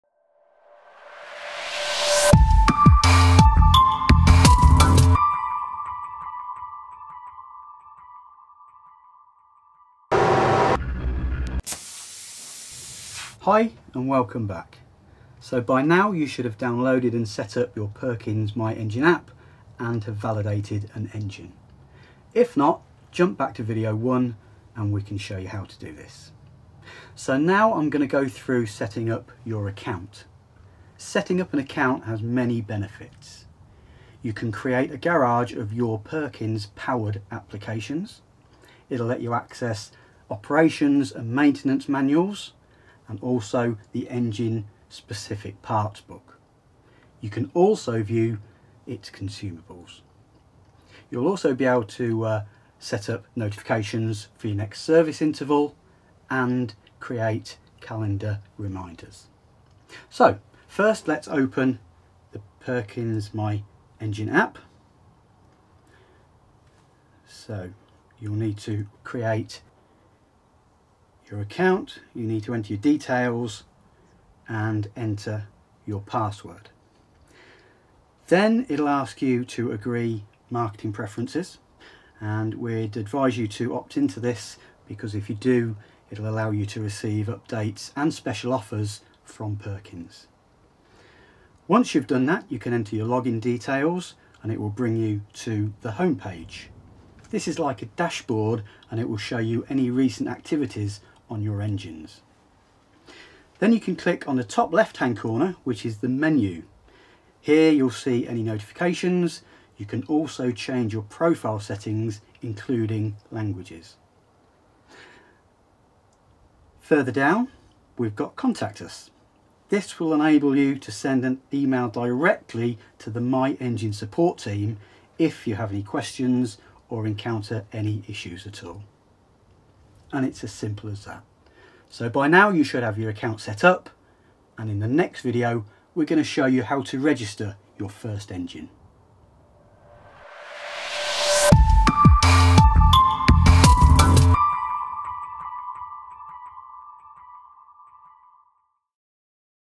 Video 2 | Perkins® My Engine App | Creating an account
You’re off to a great start, you’ve downloaded the Perkins® My Engine App and now you need to create your account. This video takes you through the process and signposts of useful features along the way. Once you have an account, you’ll gain access to service history, parts books, the operation and maintenance manual, last known location of your application and consumables specifically for the registered engine.